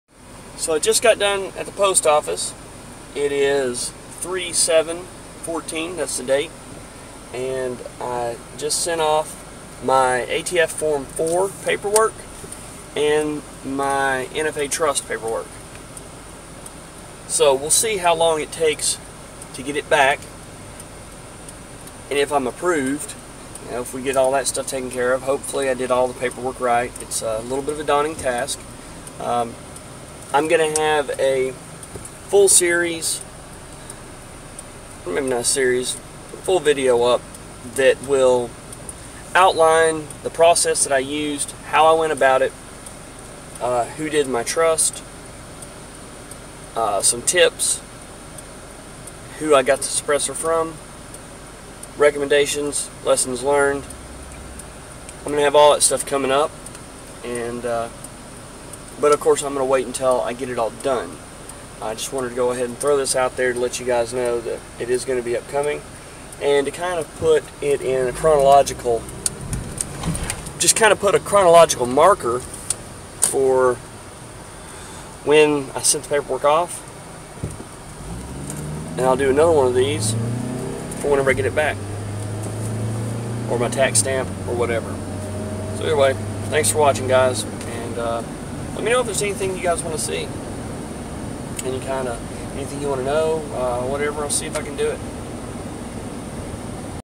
ATF form 4 and trust paperwork in the mail
I finally got my trust paperwork in ATF Form four paperwork along with the $200 check in the mail for my suppressor transfer.